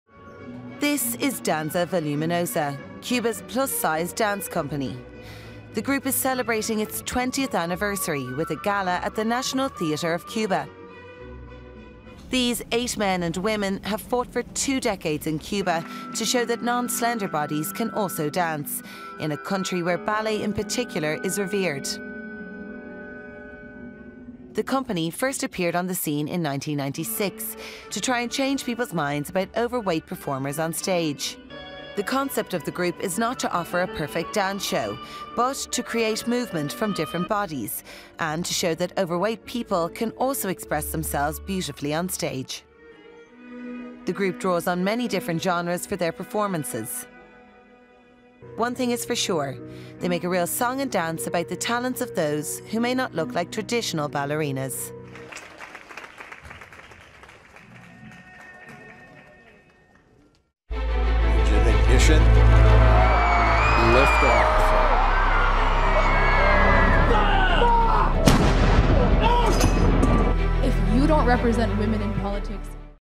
Plus-size ballet dancers in Cuba celebrate 20th anniversary
Size doesn’t matter to ballet dancers in Cuba, as plus-size troupe celebrates 20 years of performance. . Report by Sarah Duffy.